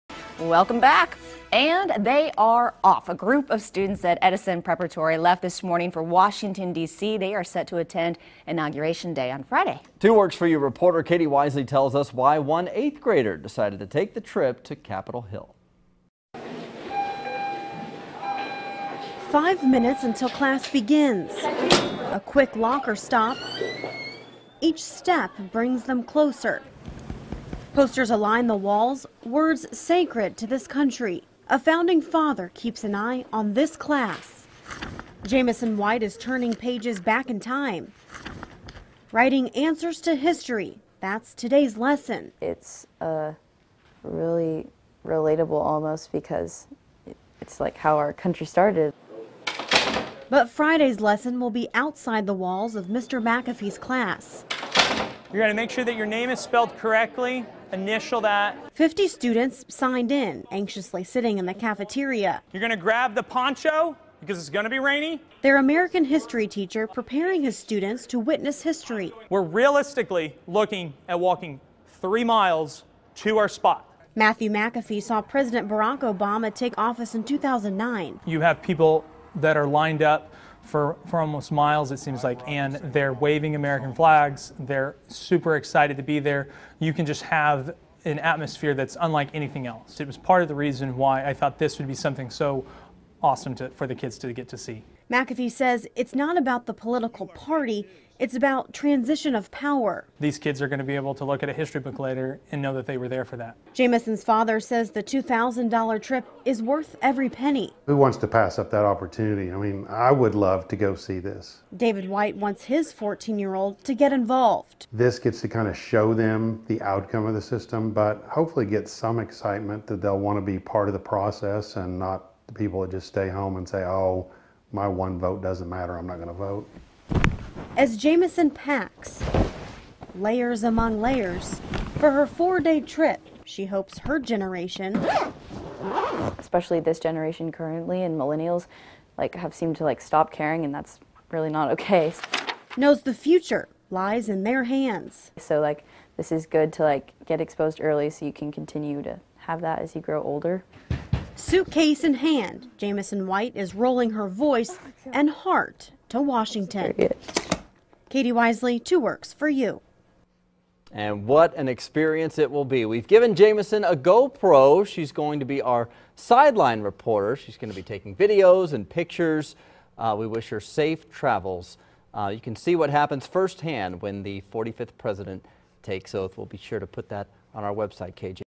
50 Edison Students Plan To Attend Inauguration Day
Plan to visit multiple museums and attend a ball.